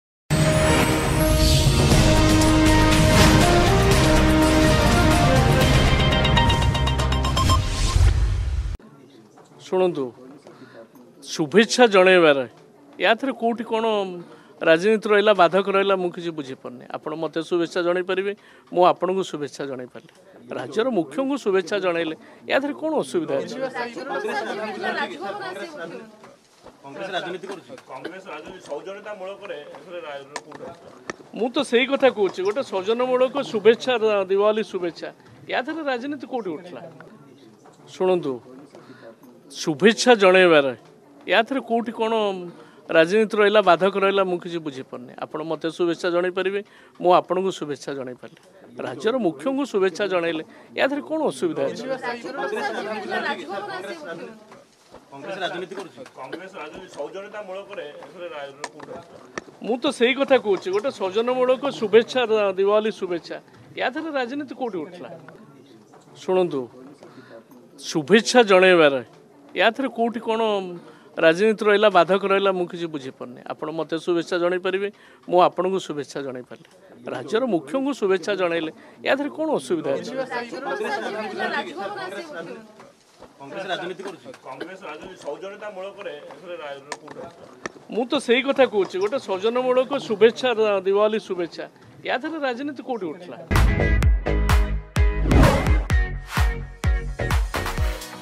Governor greeting Chief Minister is not politics: Atanu Sabyasachi Nayak || ONA KHABAR
Governor greeting Chief Minister is not politics: Atanu Sabyasachi Nayak
About Channel:
ଚ୍ୟାନେଲ୍ ବିଷୟରେ.